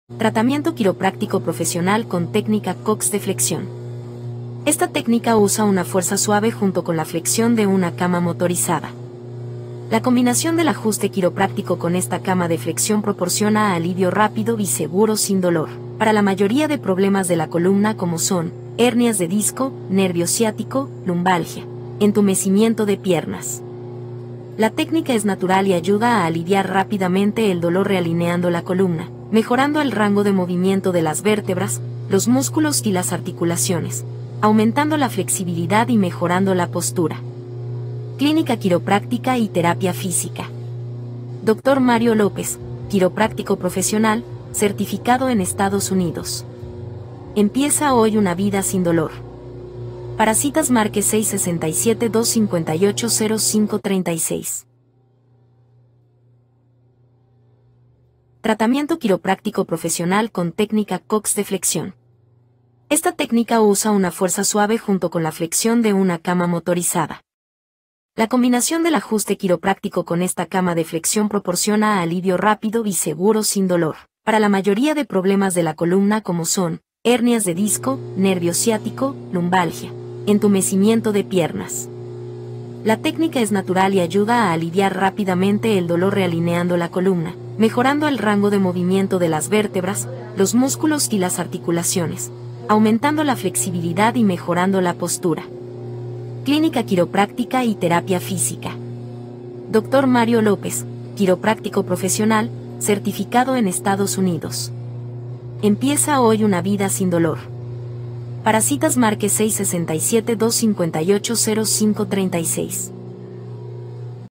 Alivio para la Ciática | Ajustes Quiroprácticos muy Efectivos
Acompáñanos mientras un experto quiropráctico explica el proceso y los beneficios del ajuste.
La ciática puede causar molestias intensas, pero un quiropráctico puede ayudar a liberar la presión sobre el nervio ciático, mejorando la movilidad y reduciendo el dolor.

Verás técnicas específicas utilizadas para realinear la columna vertebral y cómo estas pueden proporcionar un alivio efectivo y duradero. Además, compartiremos consejos sobre cómo prevenir futuros episodios de dolor ciático.